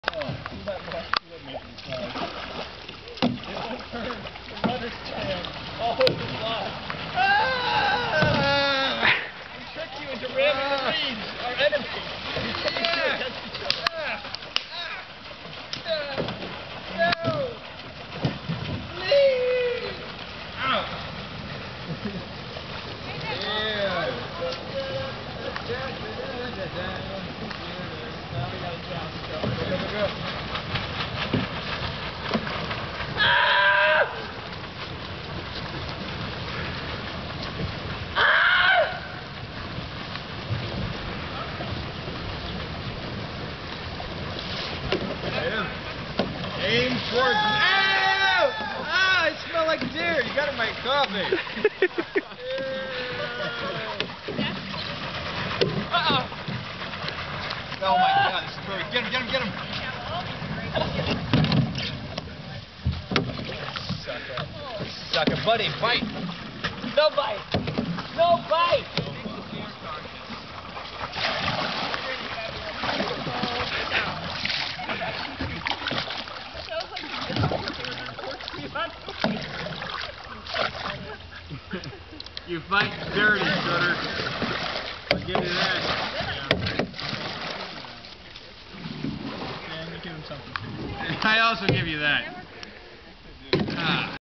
Boat Wars
These guys were feuding about which boat was better; a paddle boat or the... Other one.  It was a bit unfair, though, as it was two against one.